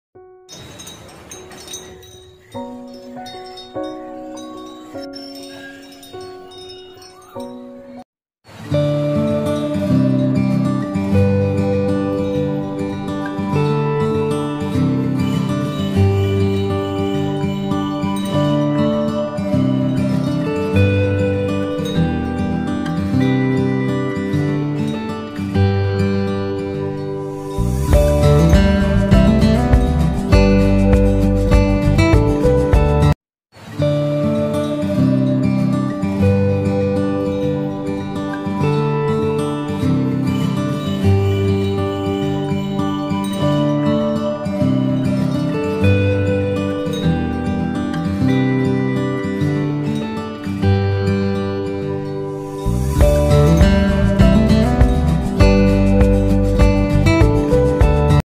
universal Message 💌 to all Dm n DF n all divine soul family 🙏song channeling hua hai plz check it 🙏
song channeling just plz check it 🙏

 God bless you all happy rehe safe rehe 🙏 meditation krte rehe ❤️

I love you all my beautiful angel's Df n Dm n my soul family ❤️

Today I am so excited n happy to share this message 💌to universe

have a feath n trust the lord 🙏

I  am Divine soul 👸